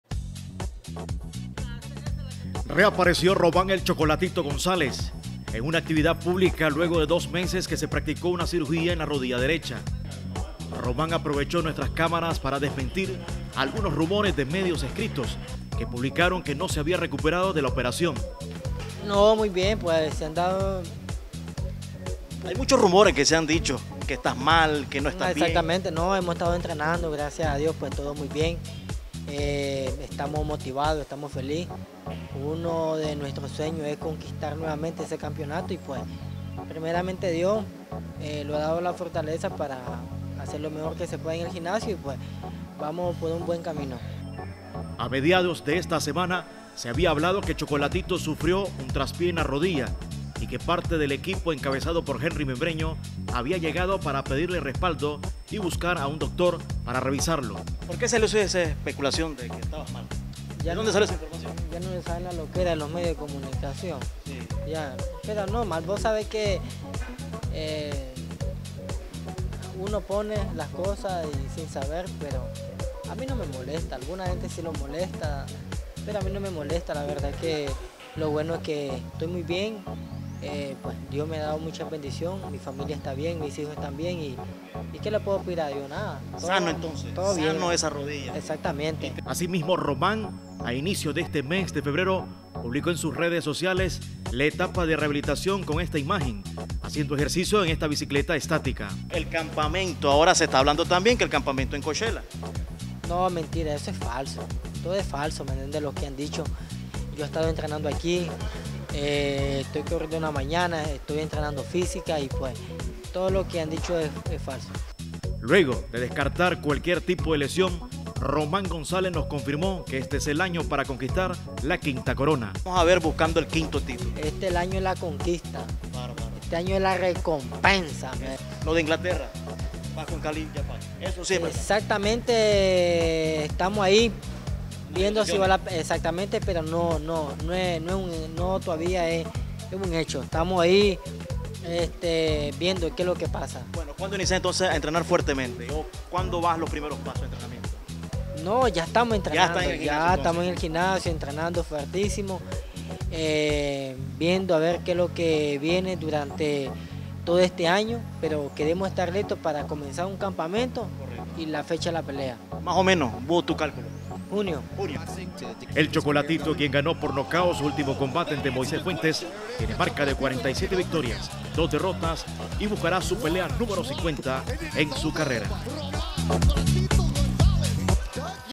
CHOCOLATITO ASEGURA QUE ESTA BIEN DESPUES DE LA OPERACION REALIZADA EN SU PIERNA DERECHA
Esta Noche :  Exclusiva
Chocolatito nos confirma que esta bien de su rodilla derecha. Desmintiendo a medios que decian que no se habia recuperado de la operación.
Espere entrevista este viernes a las 12 : 30 pm en los Fantásticos en Viva Nicaragua canal 13